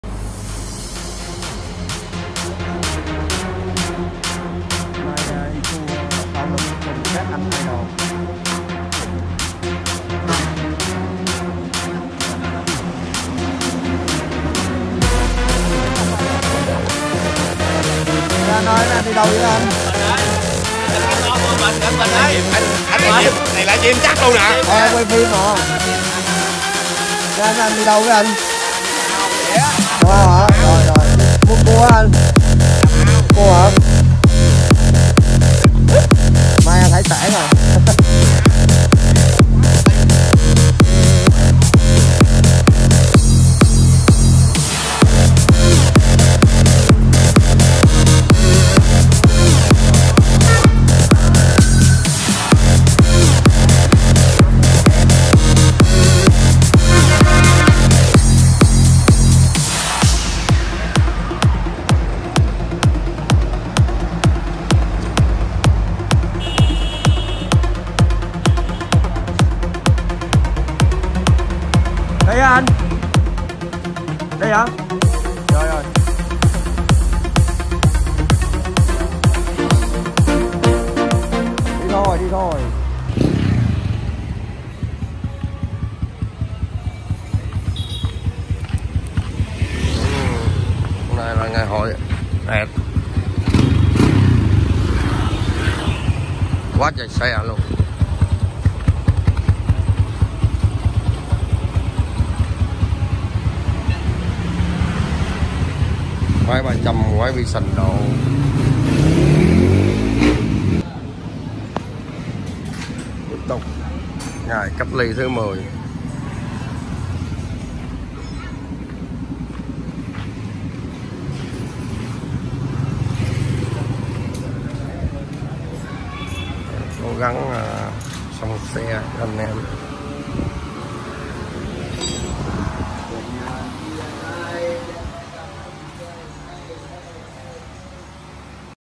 Kỉ Niệm 2 năm trước đi tua moto PKl cùng các anh idol 299 của Việt Nam Bảo Hello TBK Team
Kỉ Niệm 2 năm trước đi tua moto PKl cùng các anh idol 299 của Việt Nam Bảo Hello TBK Team 147 Tân Khai P4 Q11 Bảo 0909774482